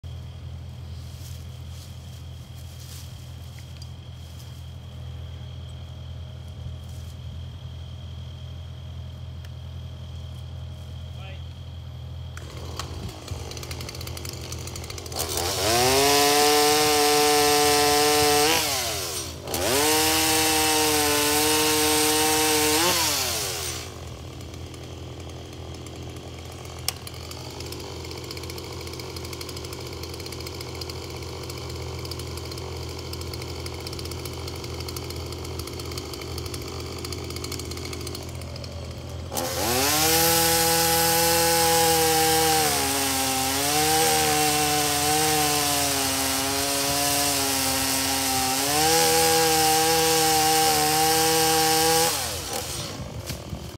When you need a boost.
Cutting tree limbs from the bucket of an excavator. Using a Stihl 170 chainsaw to cut oak tree limbs. . @cedarvalleyoutdoors Dangerous cutting.